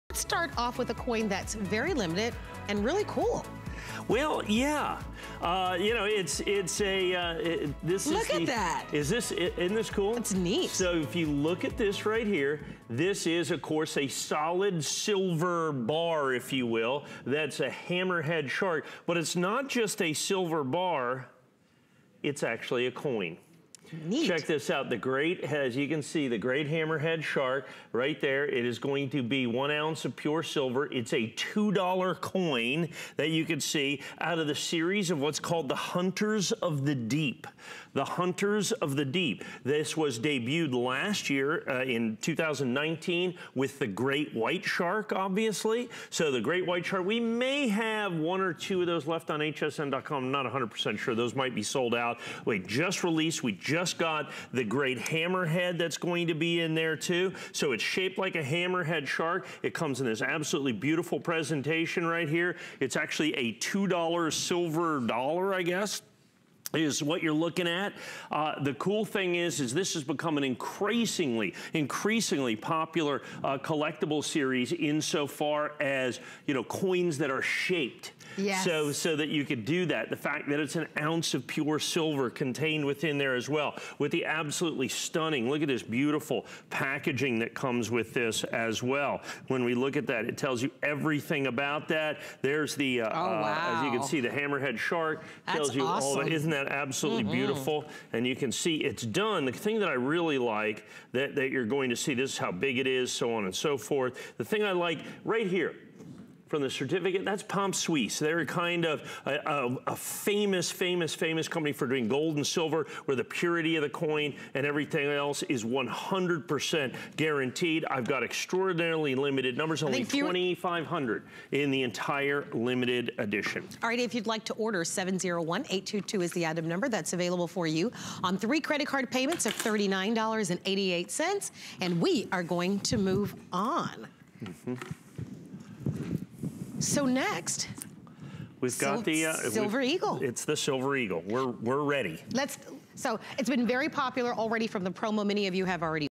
2020 Solomon Islands Hammerhead Shark Shaped 1oz. Silver...
2020 Solomon Islands Hammerhead Shark Shaped 1oz. Silver $2 Coin
It's a fun and practical way to collect precious metals. This hammerhead shark coin will be a novel conversation piece for your collection. Don't delay. It's the second in the Hunters of the Deep series, part of a mintage of 2,500 and is bound to go extinct.

    "Hammerhead Shark" silver $2 Solomon Islands coin
    Box